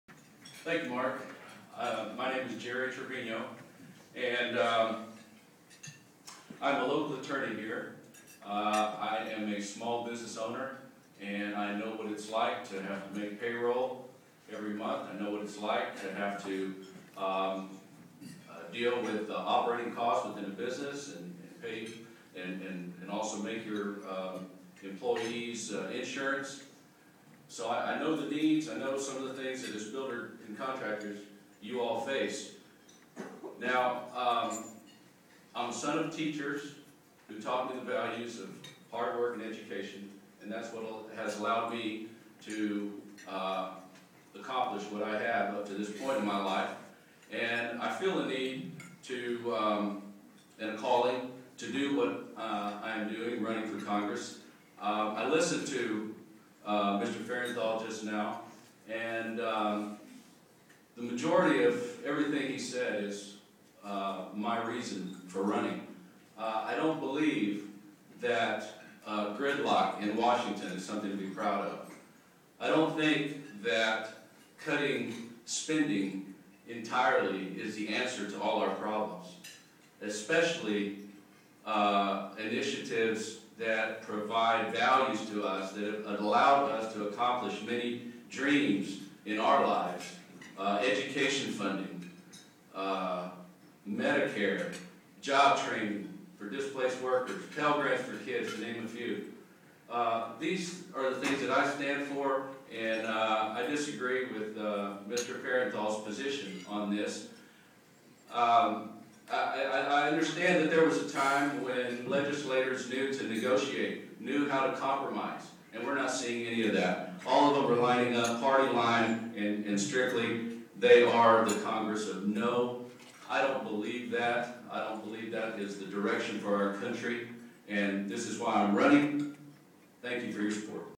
District 27 Candidate Jerry Trevino
Democrat Jerry Trevino spoke before members and guests of the Coastal Bend chapter of Associated Builders and Contractors Inc.